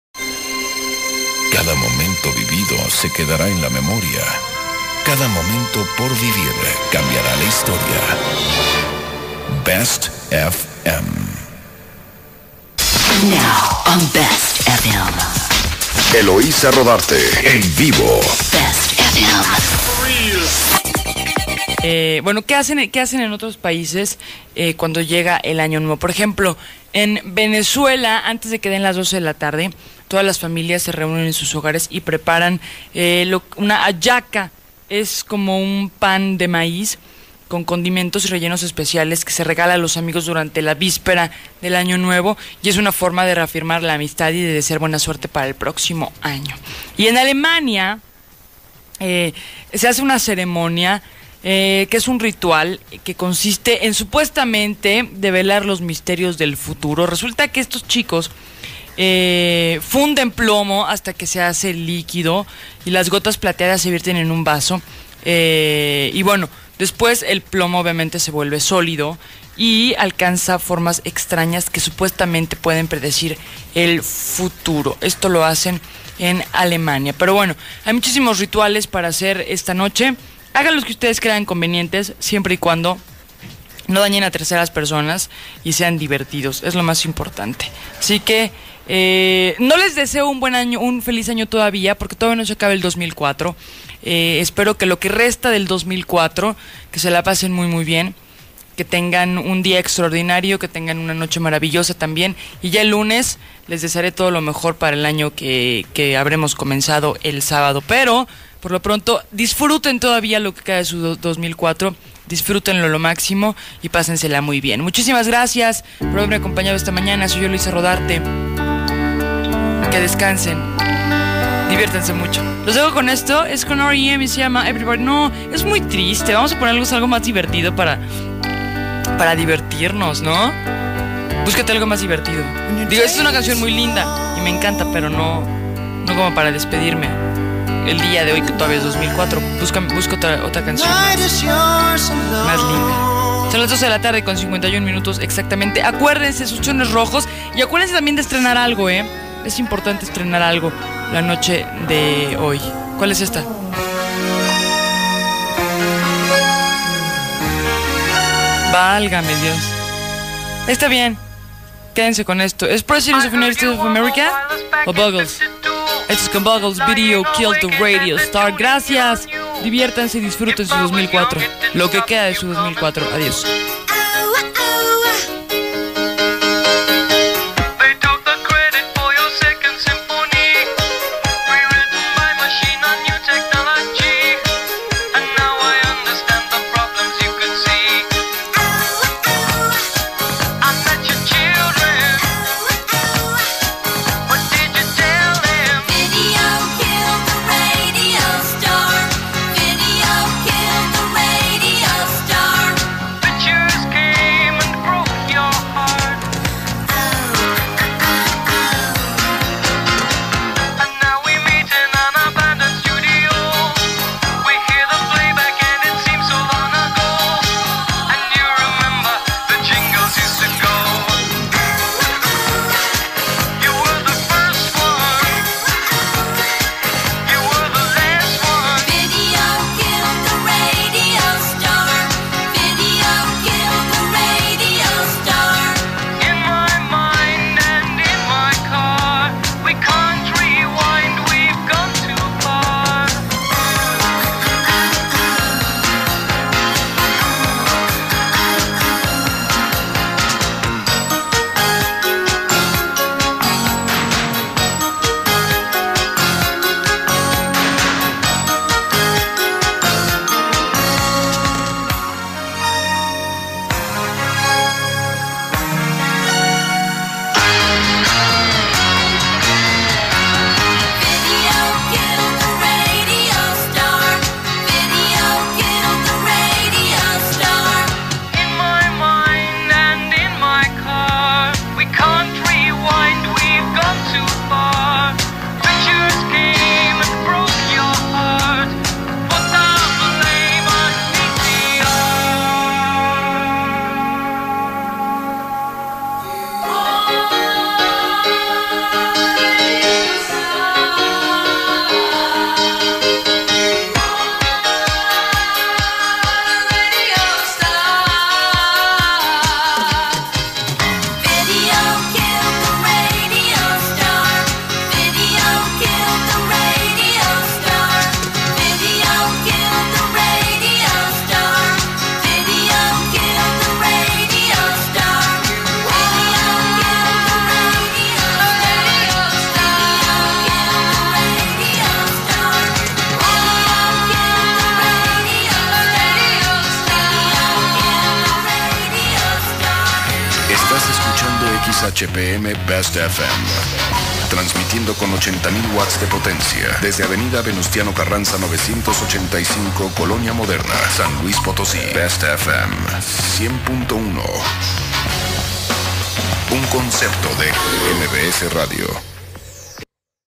Eloisa Rodarte y EL FIN de AÑO
Asi es BESTFM originalmente contaba con locución y en San luis comandaba Eloisa Rodarte Pieza esencial de bestfm 100.1 XHPM  !! aquellos buenos tiempos en que el fin de año se disfrutaban a lo grande y ahora en México se extraña un buen concepto como bestfm ! y  ahora solo unos cuantos nostalgicos estamos apegados a la internet para escucha BESTFM 103.5, esperemos que este 2010 venga con grandes sorpresas en radio !!